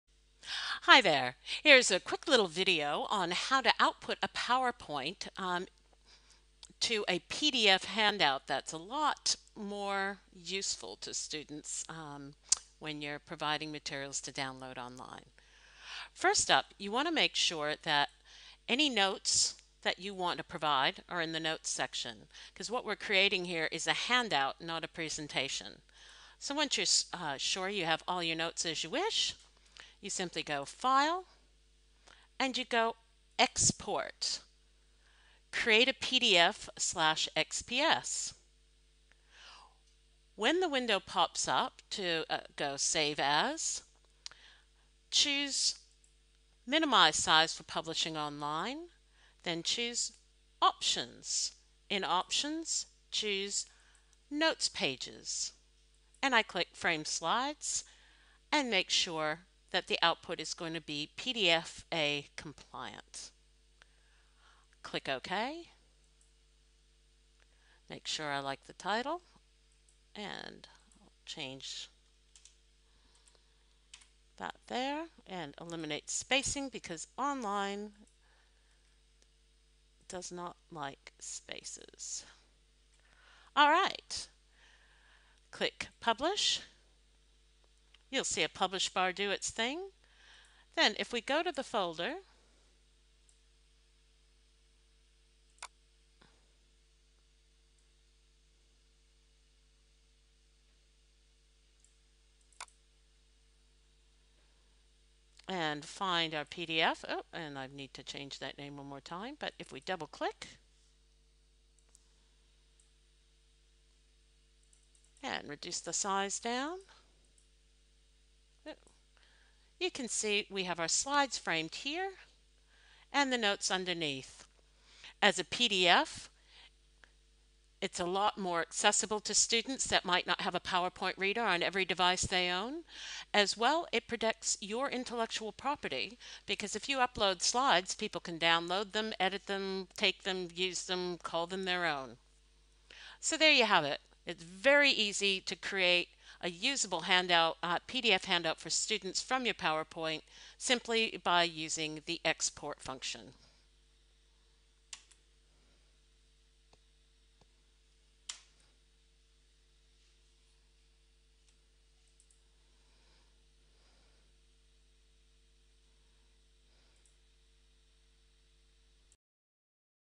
How to create PDF handouts from a Microsoft PowerPoint presentation
Uploading a PDF of your slides with note pages to your online courses protects your intellectual property and provides a far more useful resource for students than just providing slides for download.